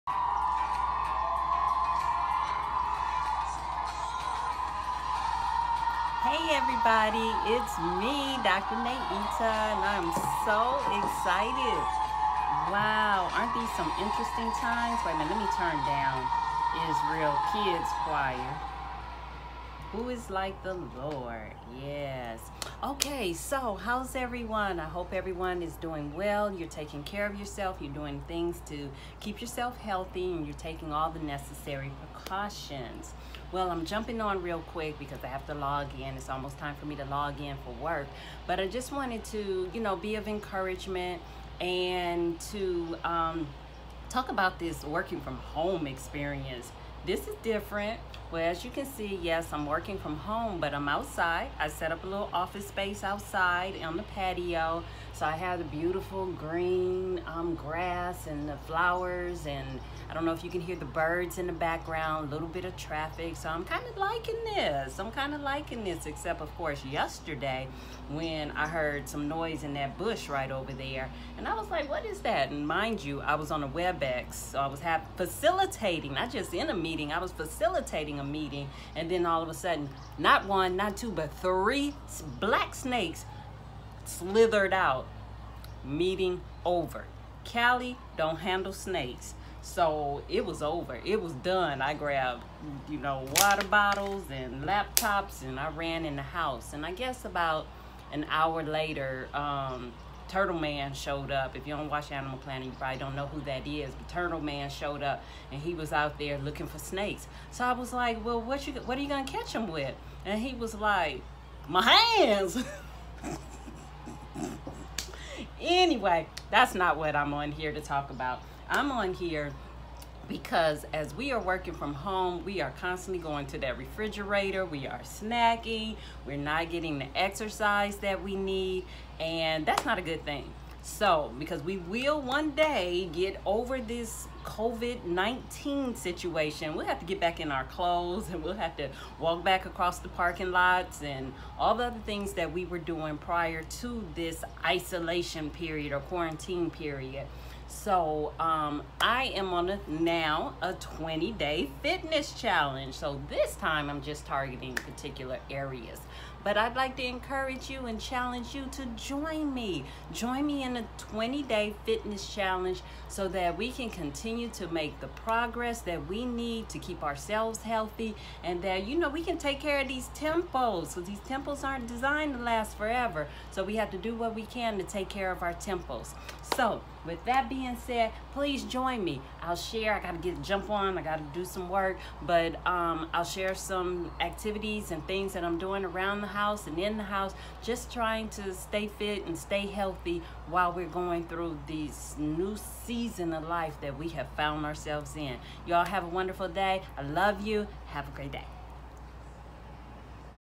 Corona 20-day Fitness Goals
This kid safe video discusses some of the challenges of staying physically fit while working from home due to the Corona Virus. Please join me for a 20-day fitness challenge. IG: @drnaita